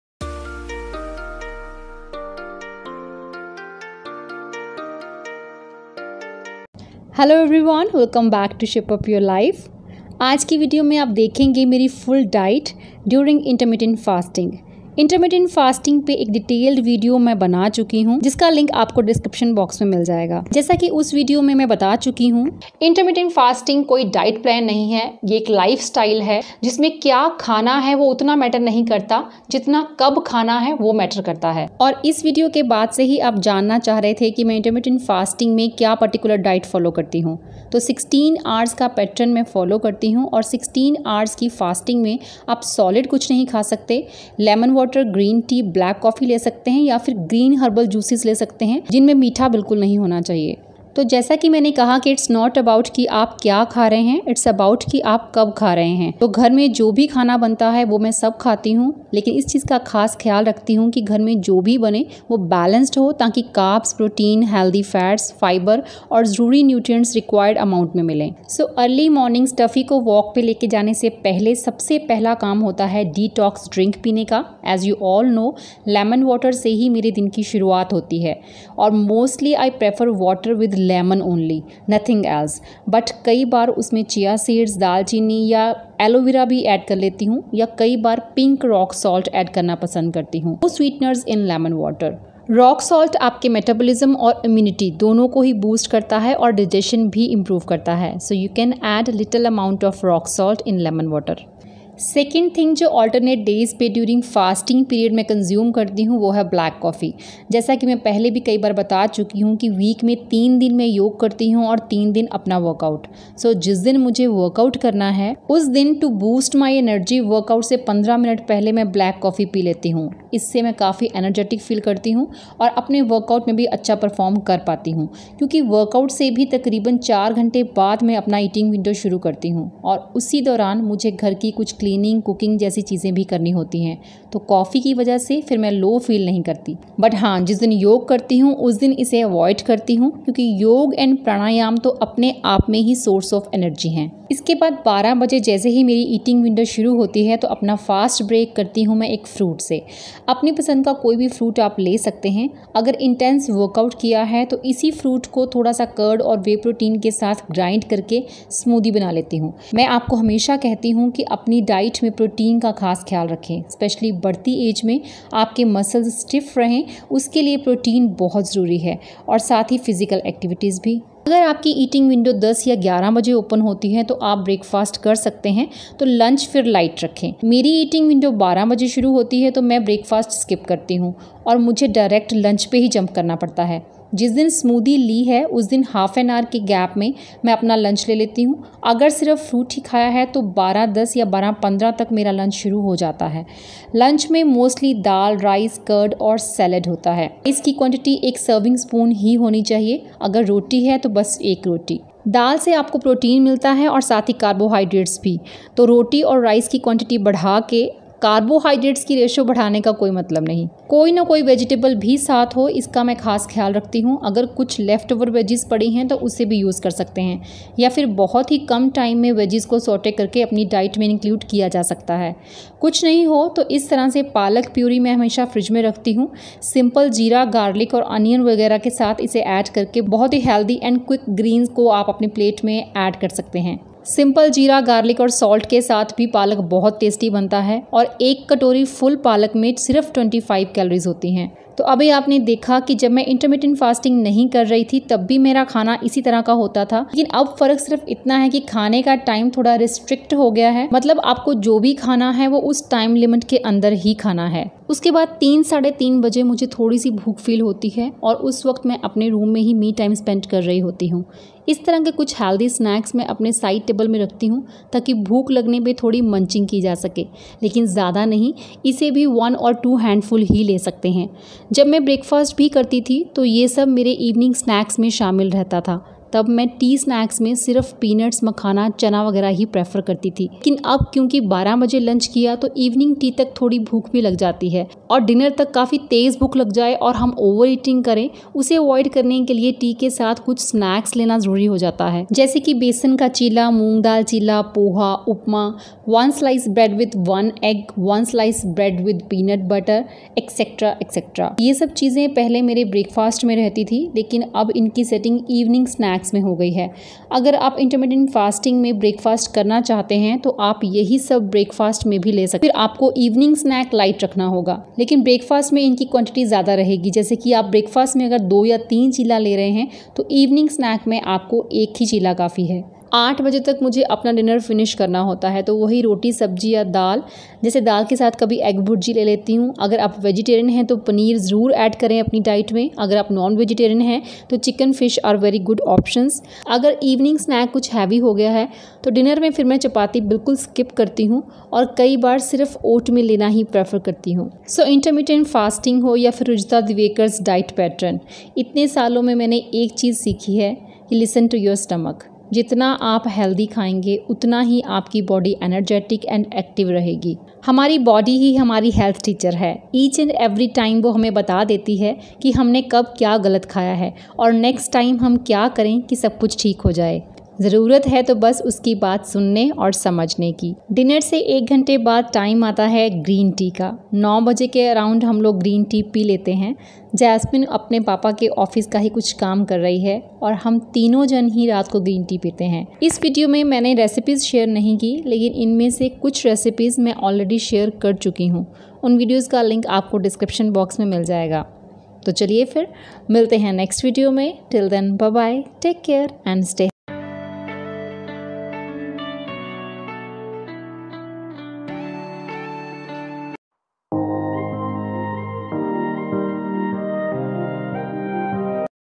What I Eat During Intermittent Fasting Easy Diet Plan
What I Eat During Intermittent Fasting|| Easy Diet Plan is a video about what i eat during my intermittent fasting routine. I hope you guys like it and find it easier to follow and understand.